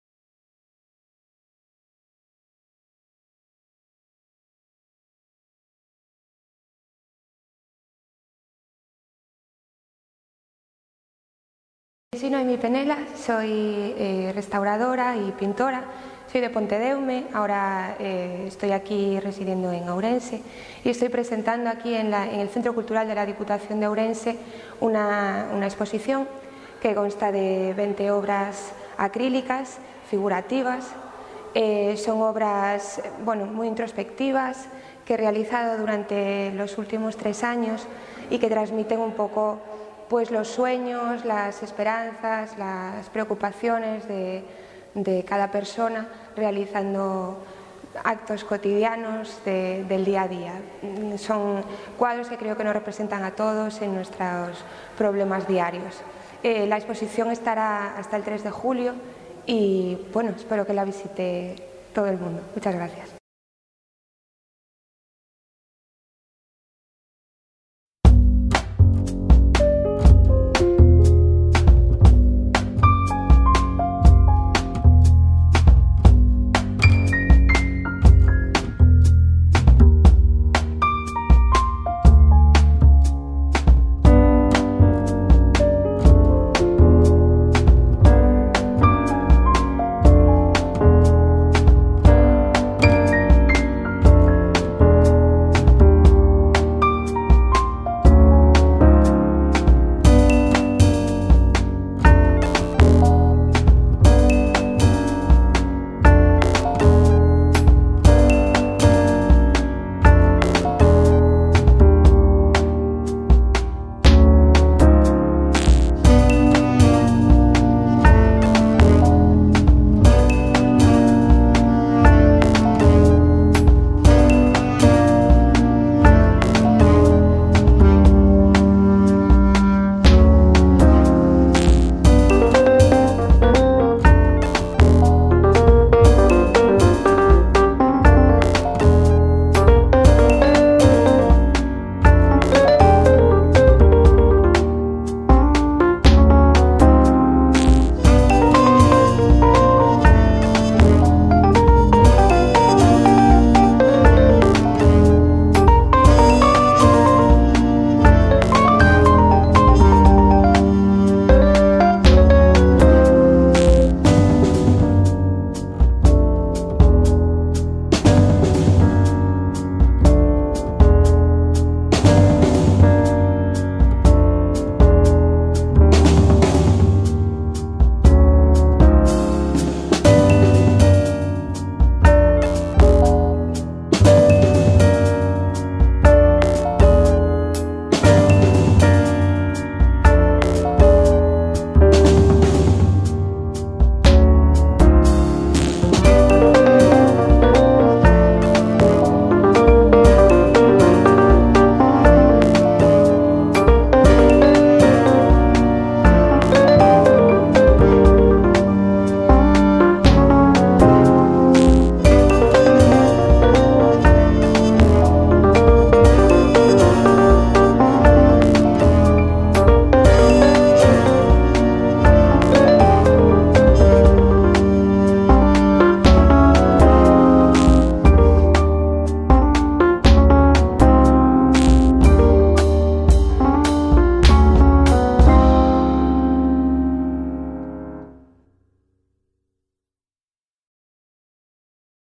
Noemi Penela. CC Deputación Ourense.
Exposición de Noemi Penela titulada "Ilustrando soños"  inaugurada o día 8 de xuño de 2011 no Centro Cultural da Deputación de Ourense.
Exposición: 8 de xuño ó 3 de xullo de 2011